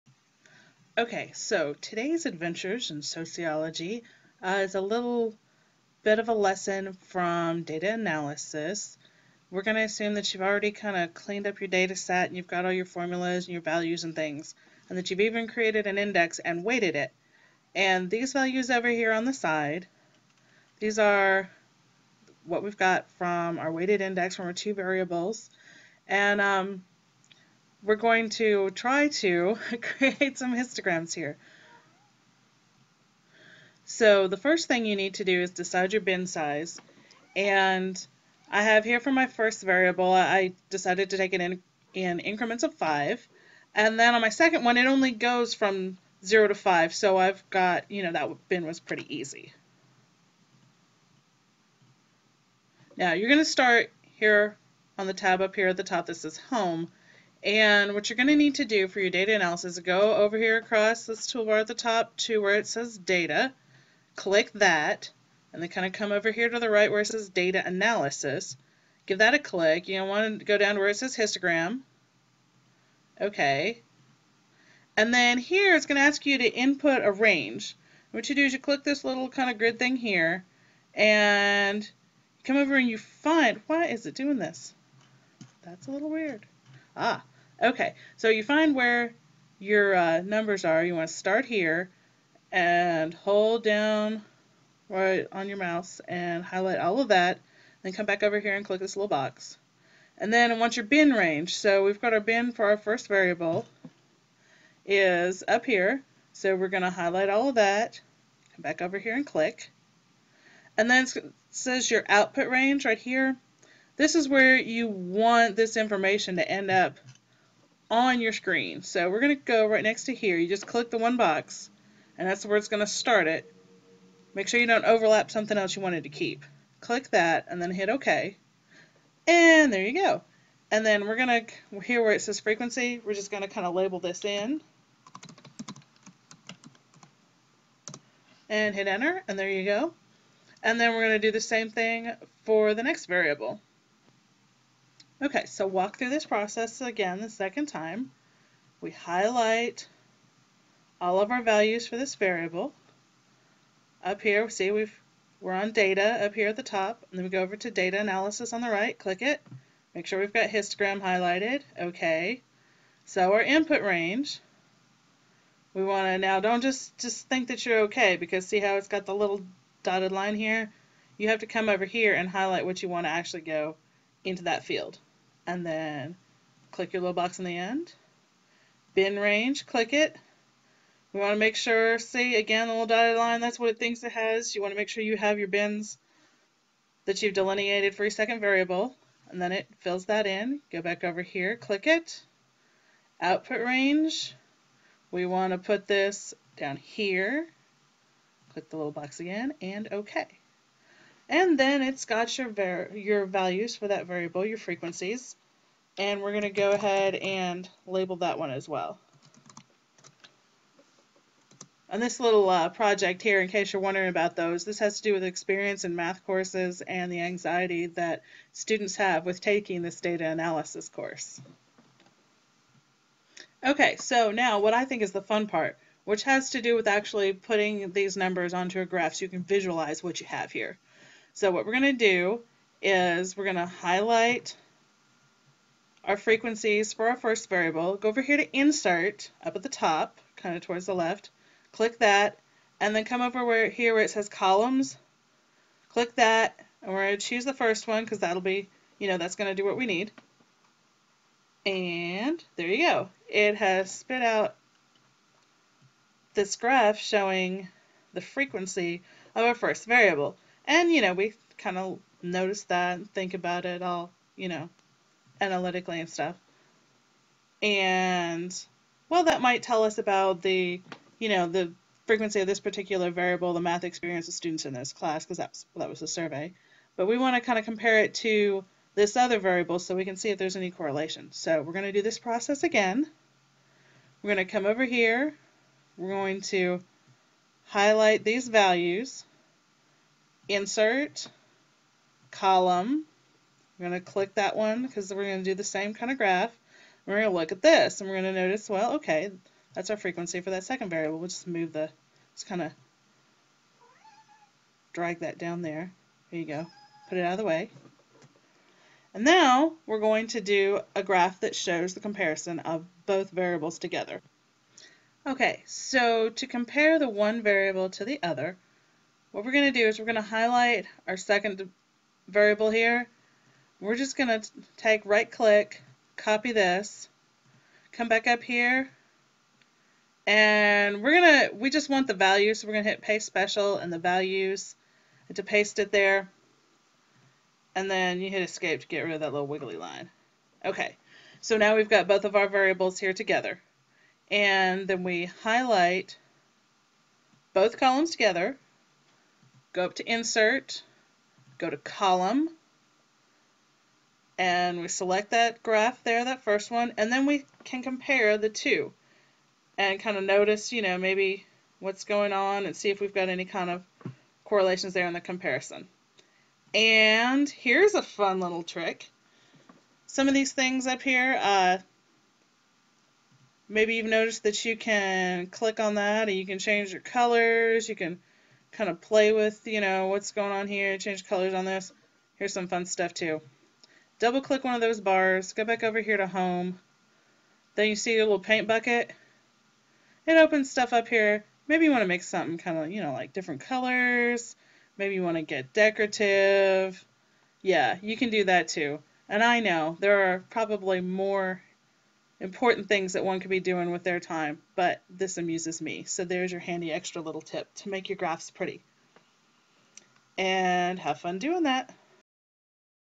Adventures in Histograms
How to create a histogram in Excel...or close enough.